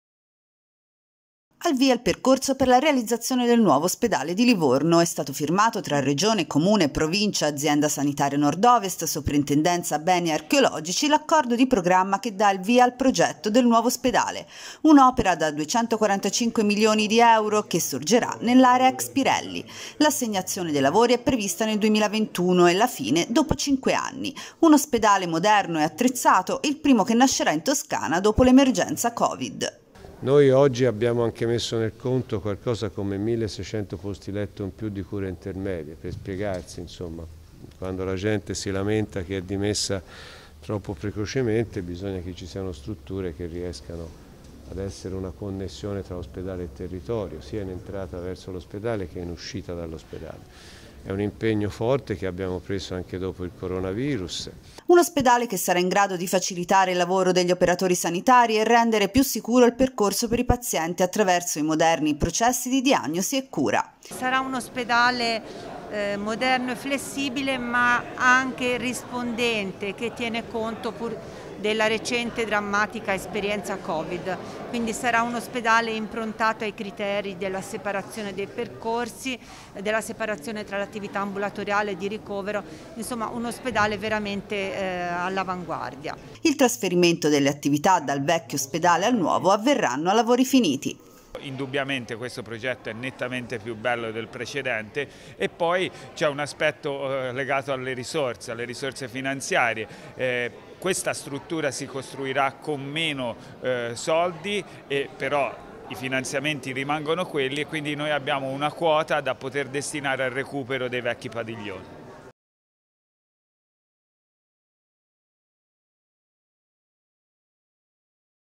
Il nuovo ospedale di Livorno
Assegnati i primi 171 milioni di euro per la realizzazione della nuova struttura sanitaria che sorgerà in una porzione del parco Pertini e nell'ex area Pirelli, progettata anche in funzione del recupero e della bonifica di un’area industriale che darà alla città un edificio funzionale ed appropriato alle esigenze dei cittadini. Il progetto di edilizia sanitaria è stato sviluppato dalla Regione Toscana assieme alla USL Toscana Nord Ovest, la Soprintendenza, Il Comune e la Provincia di Livorno con il coinvolgimento di medici ed operatori sanitari e garantirà nuovi posti dedicati alle cure intermedie in una struttura sicura, moderna e flessibile nella sua capacità di risposta alle esigenze di cura e prevenzione, anche in funzione delle esigenze emerse durante l'emergenza sanitaria causata dal Coronavirus.